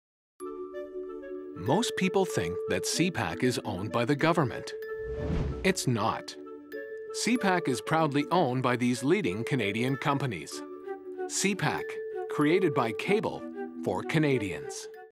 CPAC BALLOONS
Who: CPAC (The Cable Public Affairs Channel)

What: Balloons promo spot

Where: Cable and satellite specialty TV channel

This spot was produced to remind viewers that CPAC is funded by Canadian cable companies, not by the government.

2D / 3D design, animation, compositing, casting, voice record, and music.